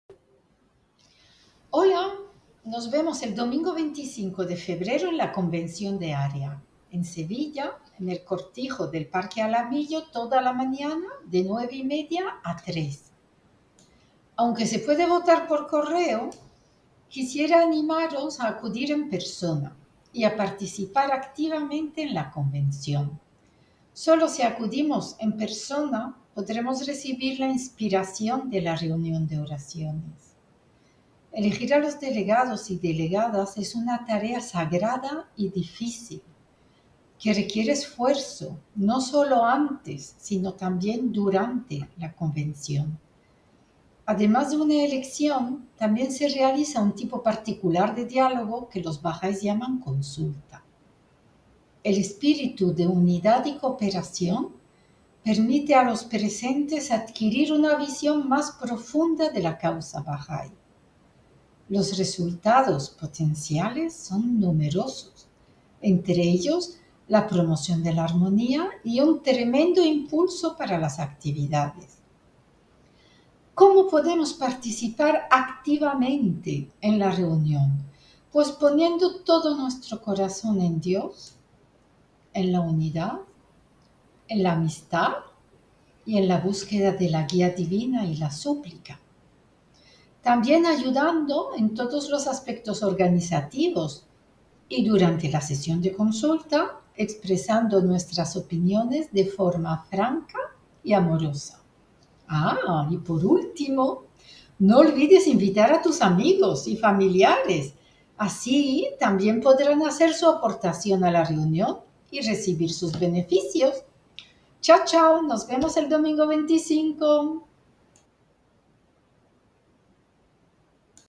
Invitación a la Convención Sevilla 2024
Animar a acudir a la Convención y a paricipar activamente en ella.